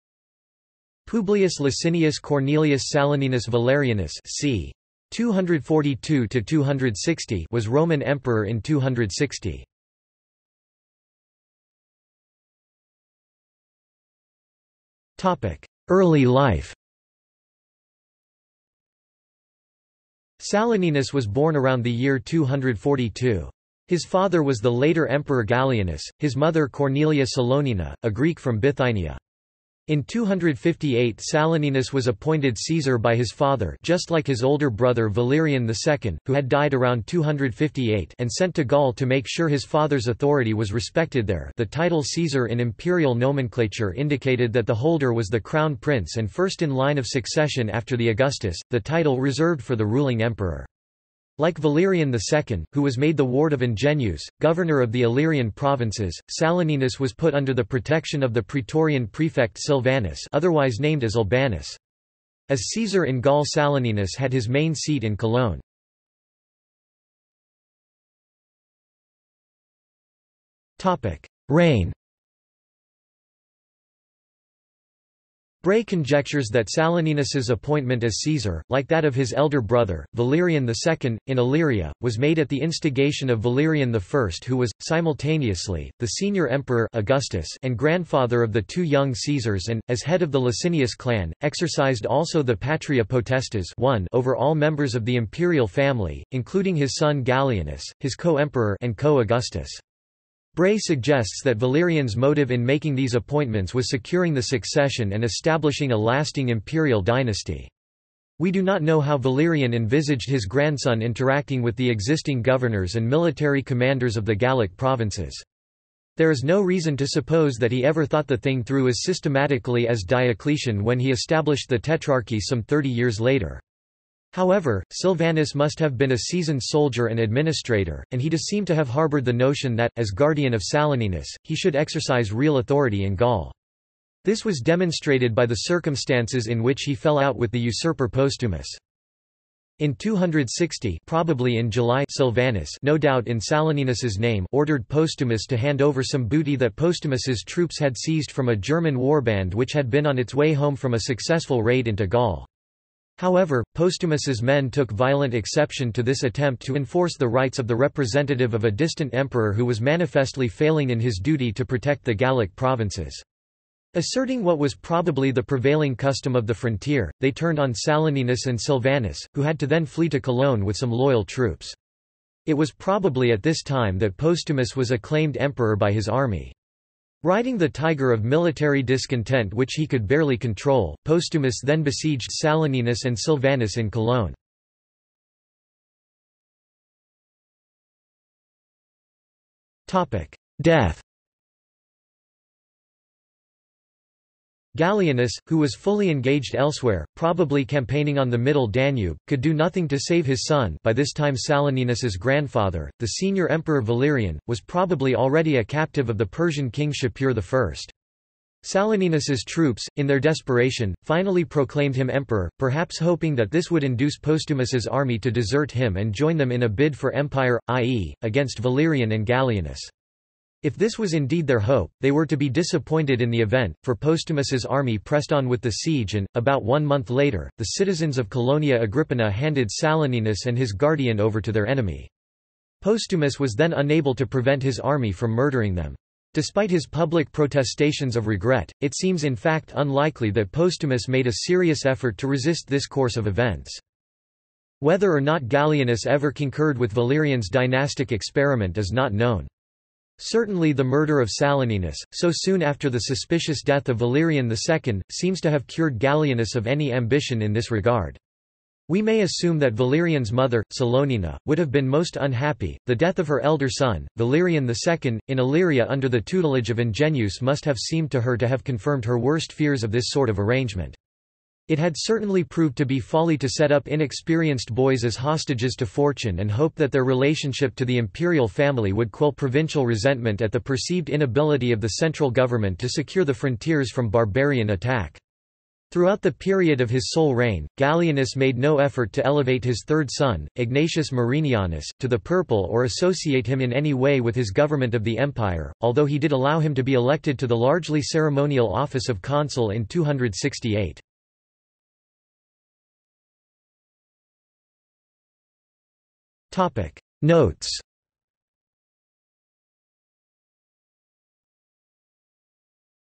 Saloninus | Wikipedia audio article
This is an audio version of the Wikipedia Article:
Saloninus

00:00:11 1 Early life
00:01:07 2 Reign
00:03:06 3 Death
00:05:02 4 Notes

- Socrates

SUMMARY
Publius Licinius Cornelius Saloninus Valerianus (c. 242 – 260) was Roman Emperor in 260.